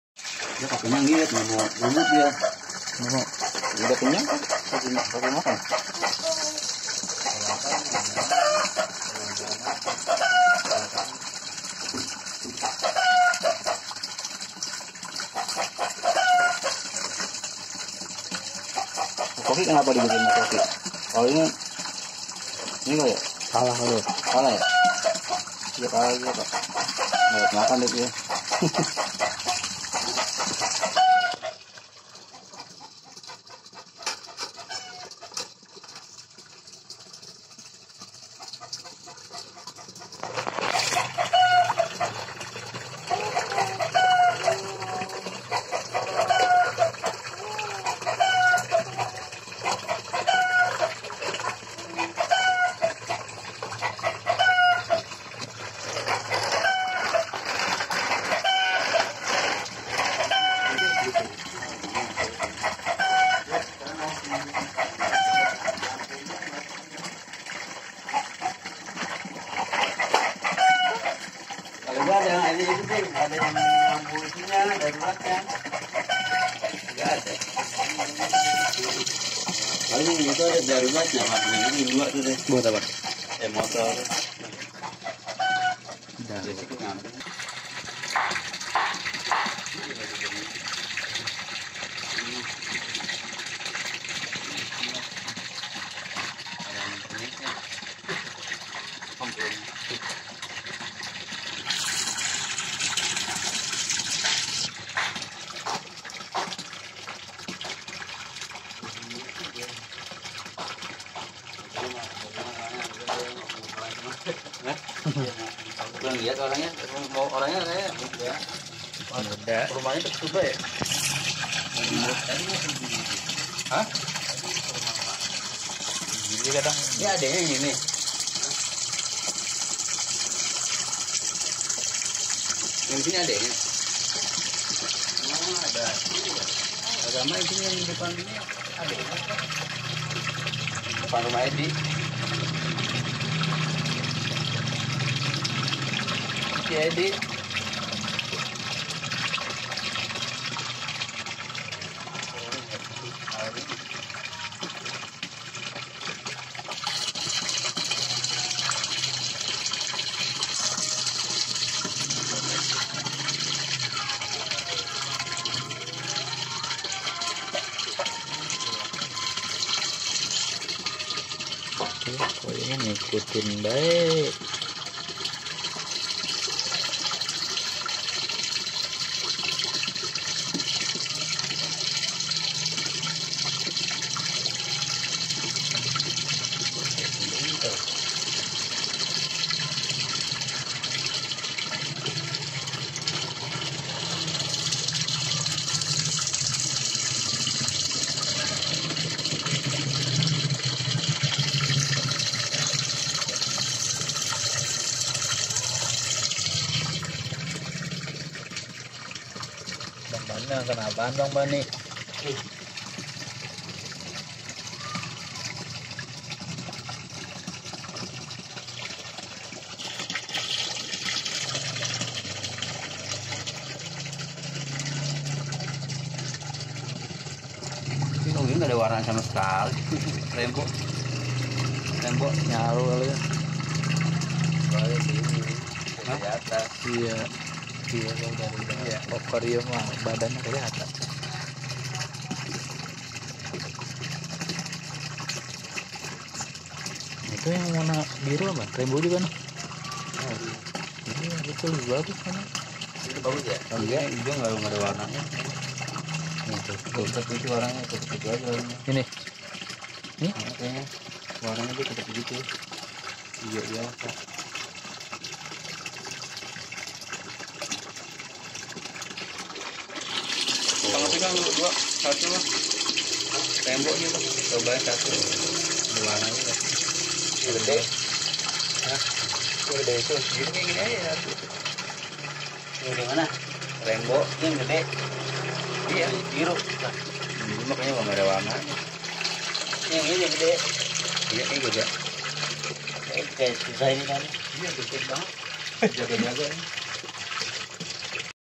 Ikan Koi Cantik dan Lucu
Pada video kali ini adalah ikan koi dengan media terpal,ada beberapa ekor ikan koi yang di pelihara di kolam ini diantaranya ada jenis Koi Showa, Shiru,